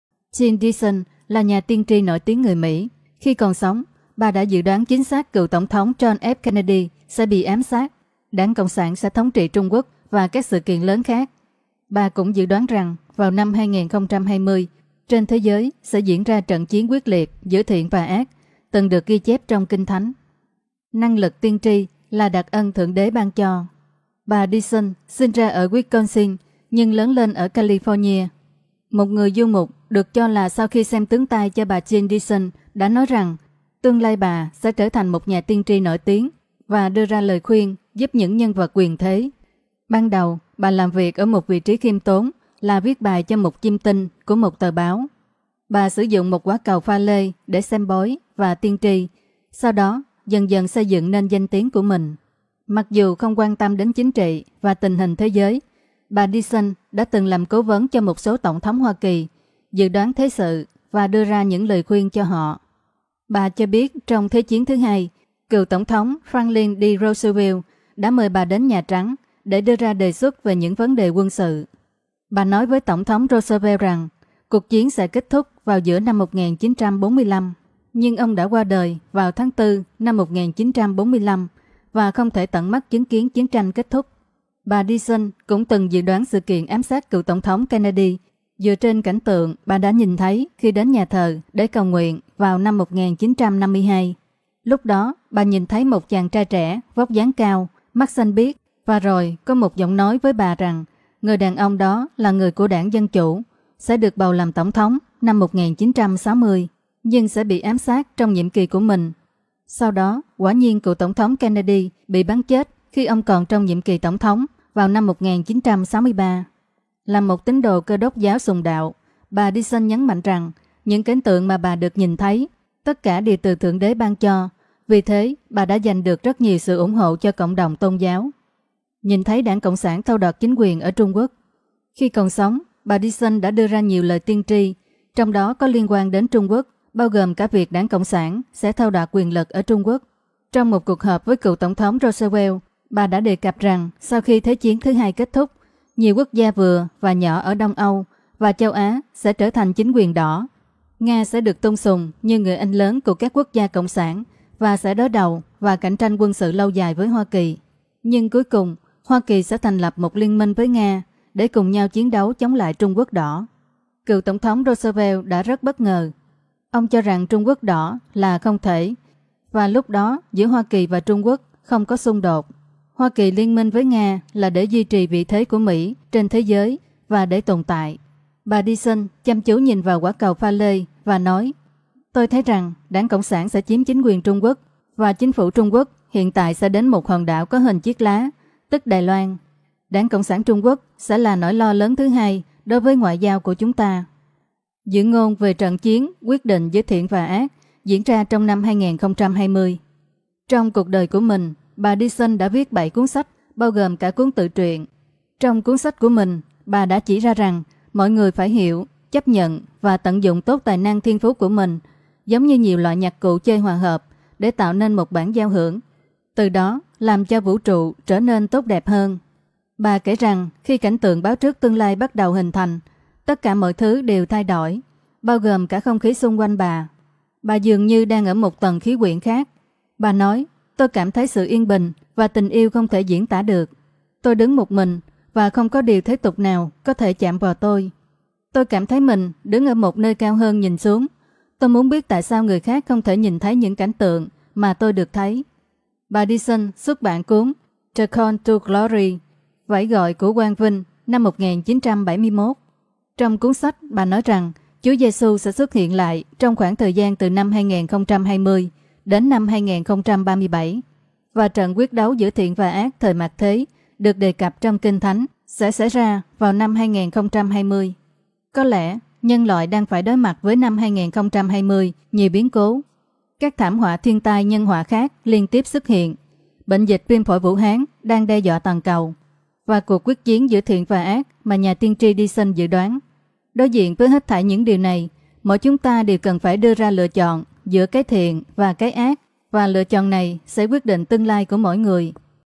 Jeane Dixon Nhà tiên tri người Mỹ: Năm 2020 sẽ diễn ra trận quyết chiến giữa Mỹ & Trung Quốc
Jeane Dixon là nhà tiên tri nổi tiếng của Mỹ, khi còn sống bà đã dự đoán chính xác cựu Tổng thống John F. Kennedy sẽ bị ám sát, Đảng Cộng sản sẽ thống trị Trung Quốc và các sự kiện lớn khác. Bà cũng dự đoán rằng vào năm 2020, trên thế giới sẽ diễn ra trận chiến quyết liệt (Armageddon) giữa thiện và ác từng được ghi chép trong Kinh thánh.